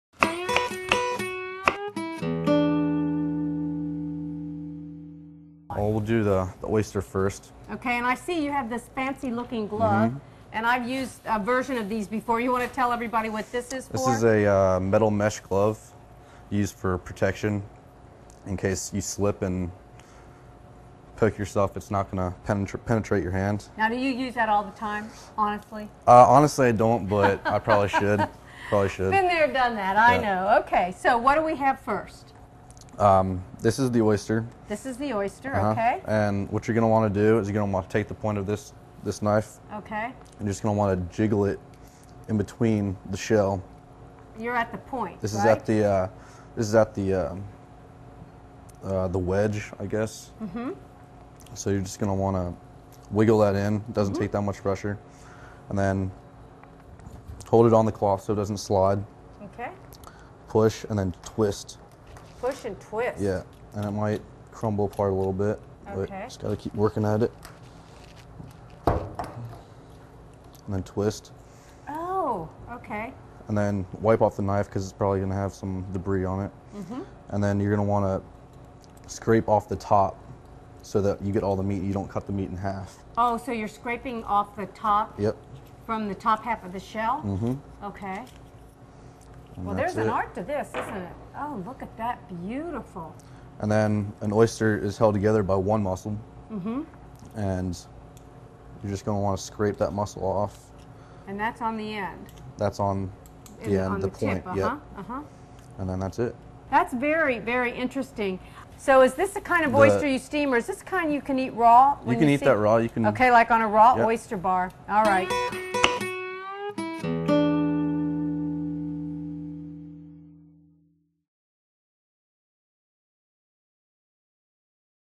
How to Shuck an Oyster: Shucking Oysters
How to shuck and Oyster: Oyster recipes aren't just on the menu when you're eating out. Shucking oysters can be done at home with the right tools  a good oyster knife and a thick towel. Oysters are available fresh at many from local fish markets and many large super markets. The key to getting good oysters is making sure you're buying them from a reputable shop.
Shucking oysters can be pretty intimidating, but with these tips you'll be able to do it as well as they do at the oyster bar.
In this video, Rita and Bryan Keegan from Keegan's Seafood, show you how to shuck oysters.